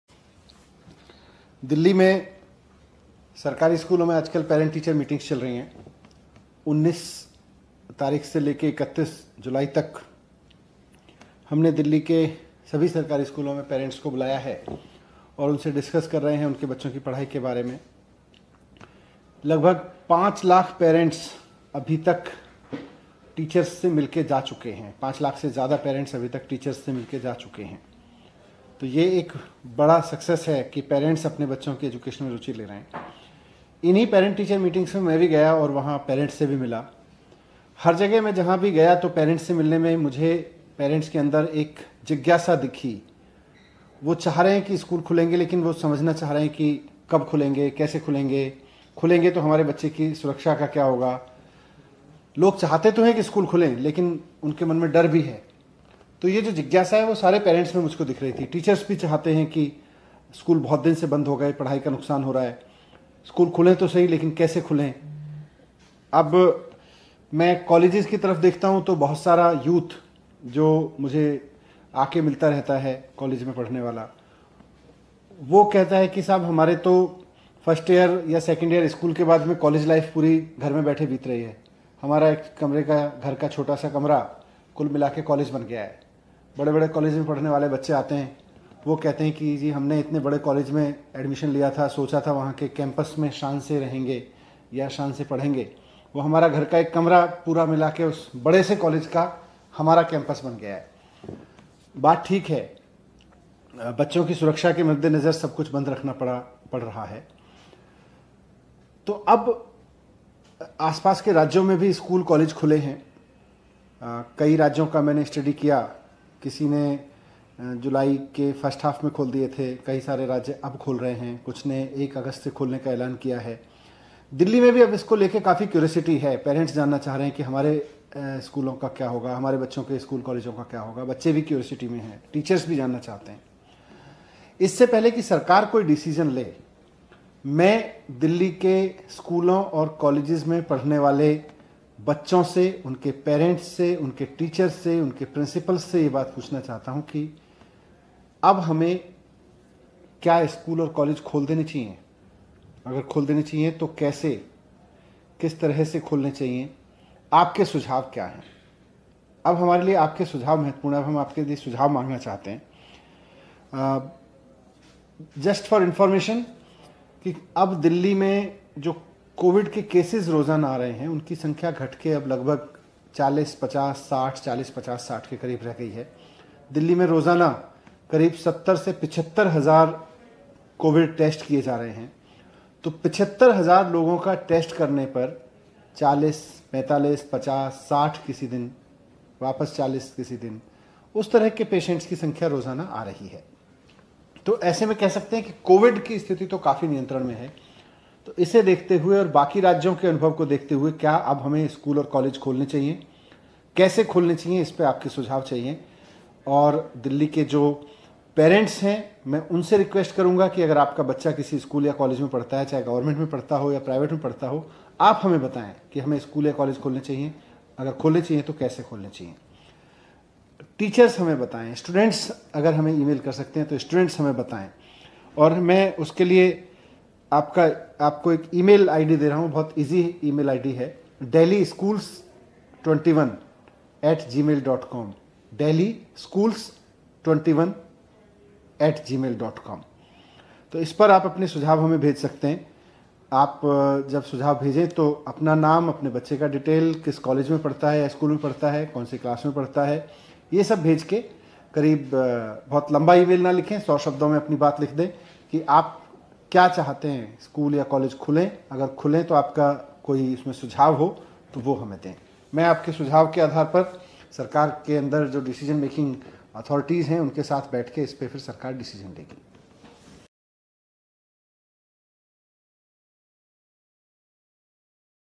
Manish Sisodia ने जनता से पूछे सवाल- क्या Delhi में School खोल देने चाहिए | वनइंडिया हिंदी
Now Delhi is on the path of unlock. Almost everything has been opened in the whole of Delhi. Meanwhile, most of the students of the capital want to know when schools will open in Delhi. Today Deputy CM and Education Minister Manish Sisodia sought their opinion from the students, their parents and teachers regarding the opening of the school. Manish Sisodia also issued an email to send suggestions

अब Delhi unlock की राह पर है. लगभग पूरी दिल्ली में सबकुछ खोल दिया गया है. इस बीच दिल्ली में स्कूल कब खुलेंगे ये राजधानी के ज्यादातर students जानना चाहते हैं. आज राज्य के Deputy CM और Education Minister Manish Sisodia ने स्कूल खोलने को लेकर छात्रों, उनके माता-पिता और शिक्षकों से उनकी राय मांगी. Manish Sisodia ने सुझाव भेजने के लिए एक ईमेल भी जारी की